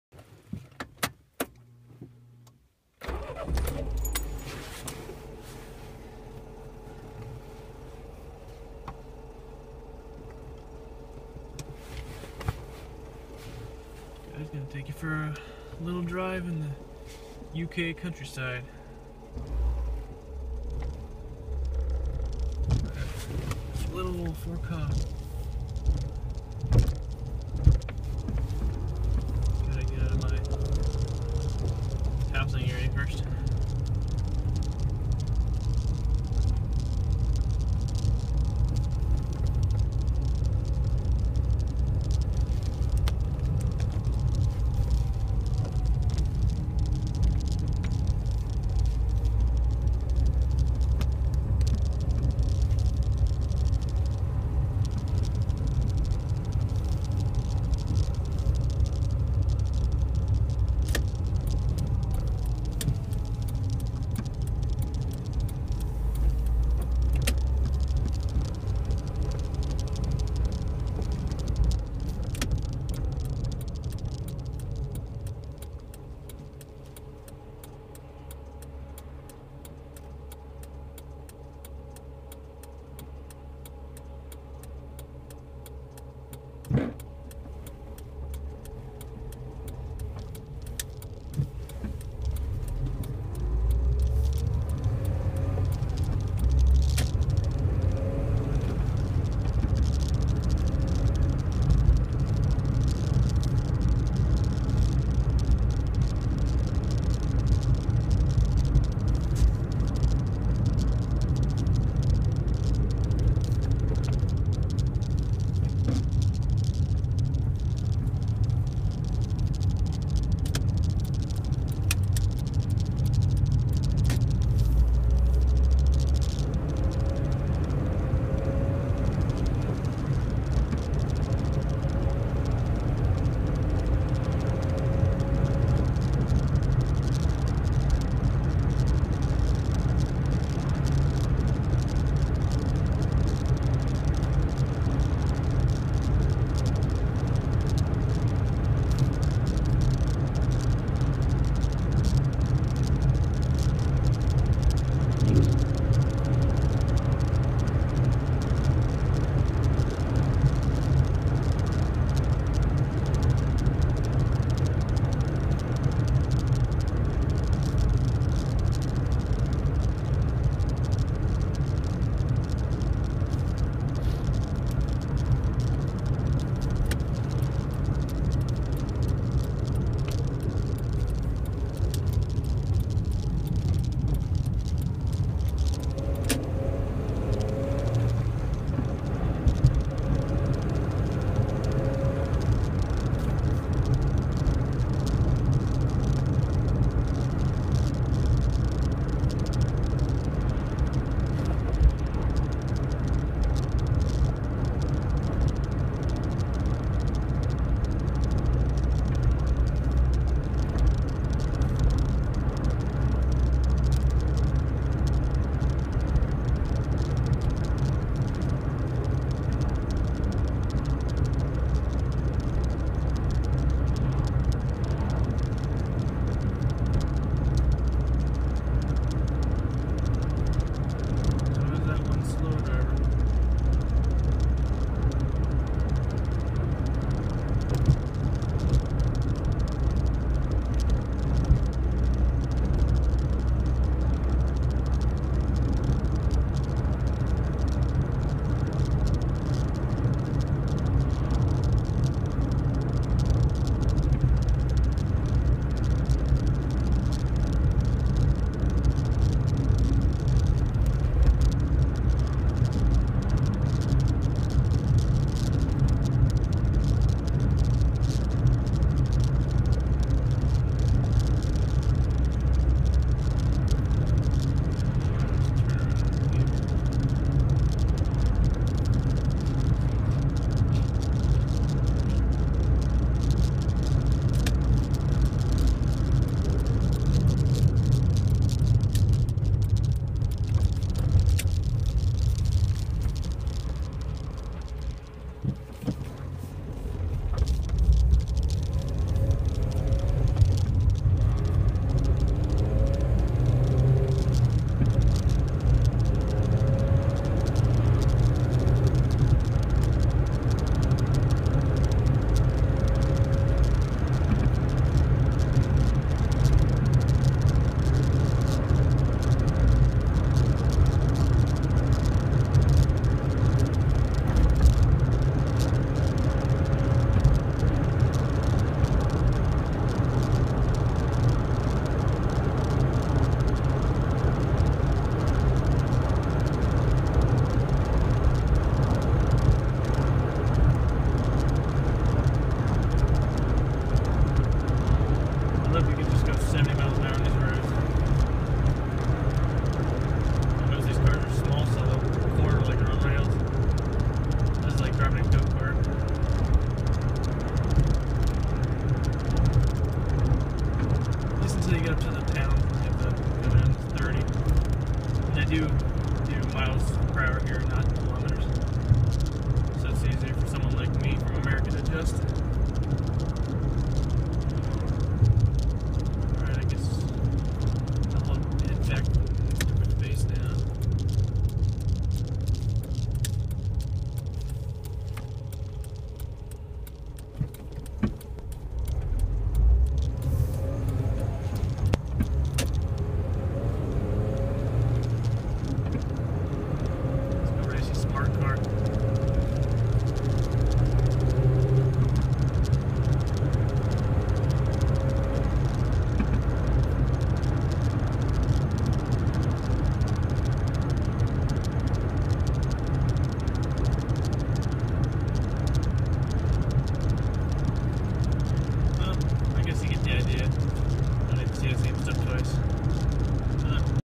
Driving around Lakenheath, UK
Quick drive round lakenheath in my ford ka